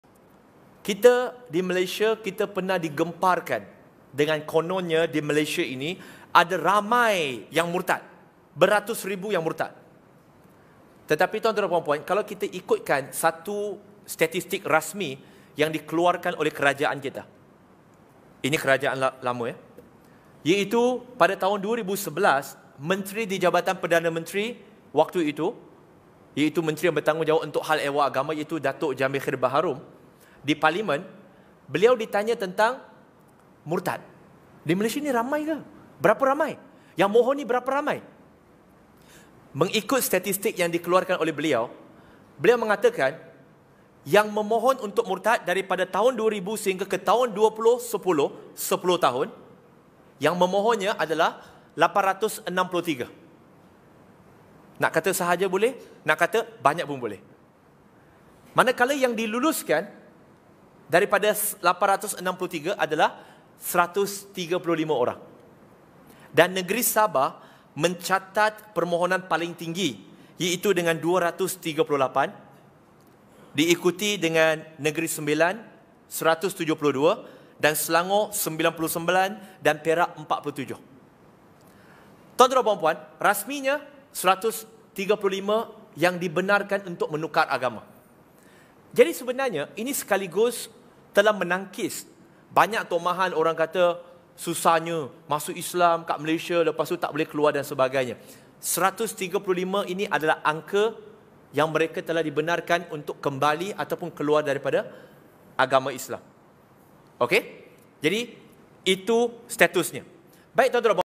Pendedahan yang mengejutkan!!! Berapa Ramai Muslim Yang Murtad di Malaysia???
Video Penuh :
Bro. Firdaus Wong Wai Hung || Murtad : Realiti Atau Fantasi